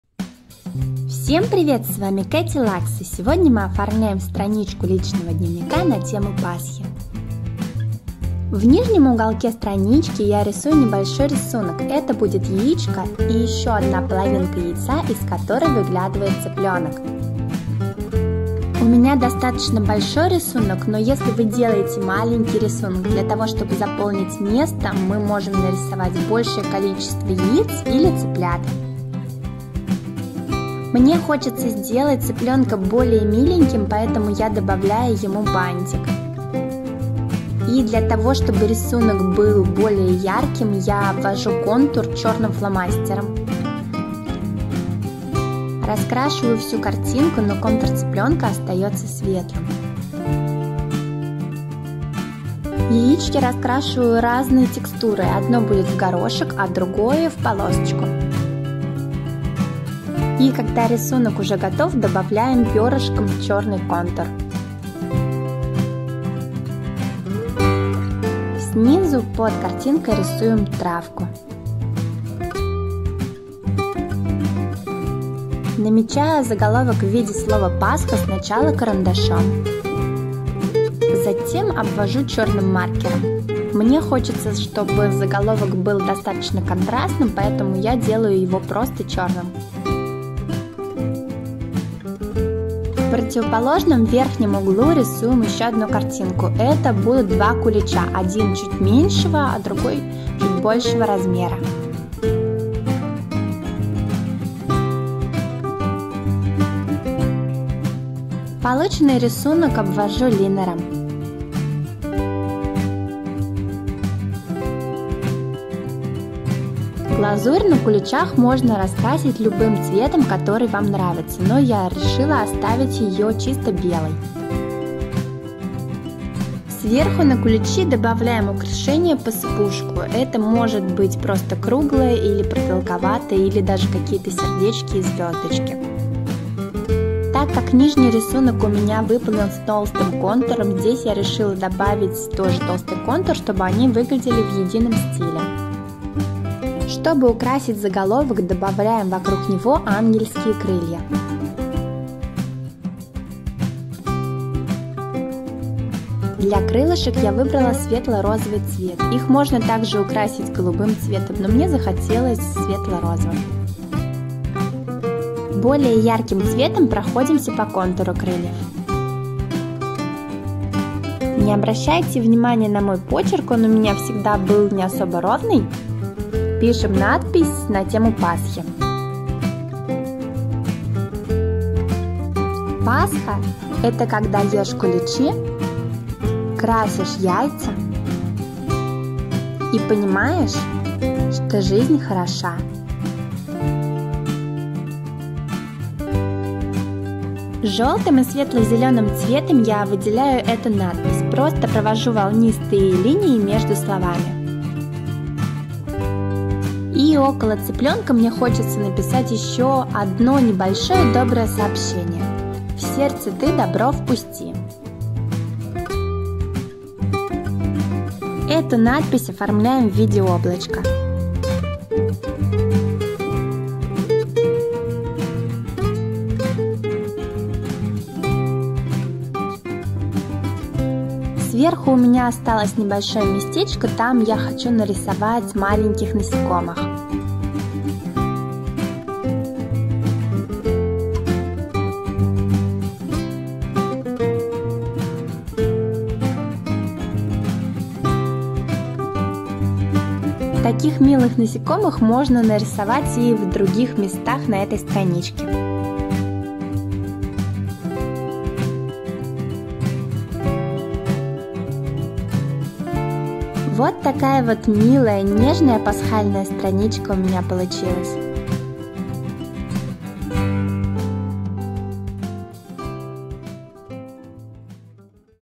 Katy Laks личный дневник/лд оформление на тему Пасхи /Пасхальная страничка
Скоро Пасха, и по вашим просьбам делаю оформление личного дневника.
У меня появилась милая идея на тему пасхи, Пасхальная страничка лд.
Рисунки на пасху можно найти у меня на канале, или оставить оформление как у меня. Обязательно рисуем пасхальные яйца. Как символ пасхи.